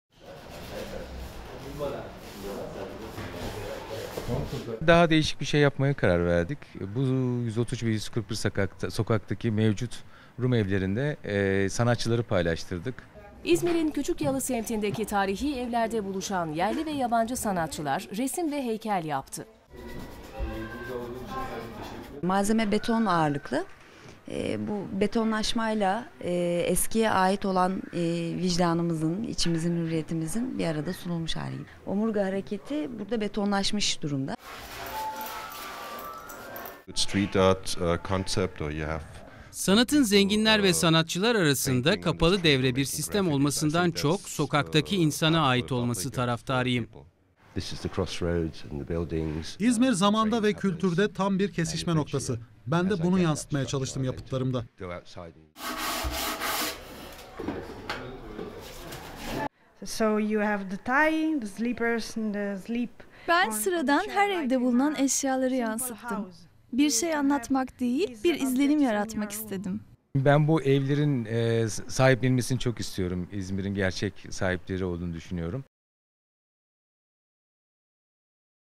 ESKİ EV YENİ SANAT 2012
Yerli ve yabancı sanatçılar, İzmir'in eski evlerini gündeme getirmek amacıyla düzenlenen "Eski Ev Yeni Sanat" etkinliğine katıldı. 12'si Türk, 8'i yabancı ressam ve heykeltraşlar, İzmir'in cumbalı eski evlerinde konakladı ve yeni eserler üretti...
Etkinliğin ana fikri herkes için sanat idi.